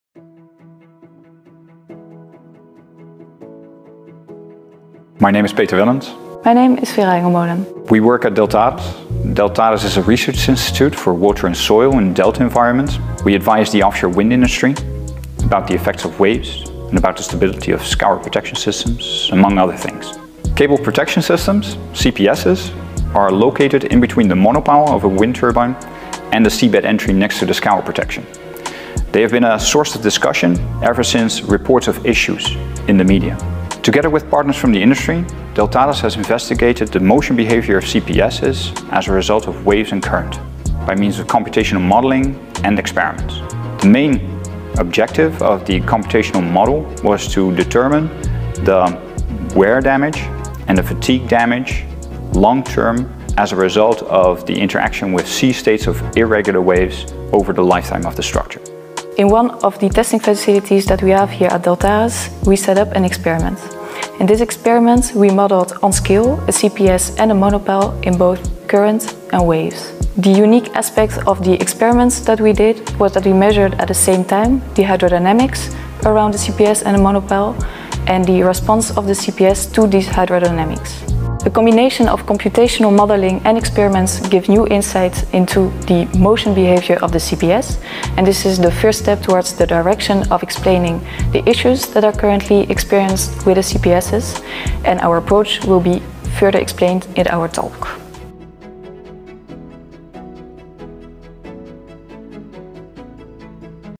Peter Wellens, Vera Hengelmolen,Deltares, 3rd Annual Submarine Power Cable and Interconnection Forum
MEET OUR SPEAKERS FOR 3RD ANNUAL SUBMARINE POWER CABLES & INTERCONNECTION FORUM

Peter Wellens, Specialist, Deltares and Vera Hengelmolen, Jr. Researcher/Advisor - Loads on Offshore Structures, Deltares will be speaking at our event, 3rd Annual Submarine Power Cable and Interconnection Forum.

The event aims to address the latest trends, challenges, and opportunities in this rapidly evolving industry, and provide a platform for networking and collaboration among participants.